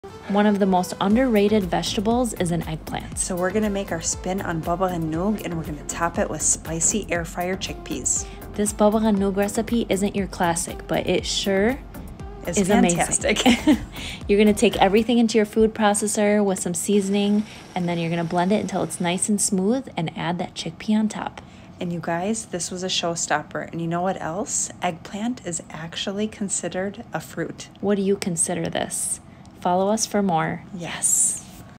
Make this Baba Ghanouj, it’s amazing!
Did you know that eggplant is considered a fruit? It’s one of the most underrated foods! Try our spin on a classic baba ghanouj recipe!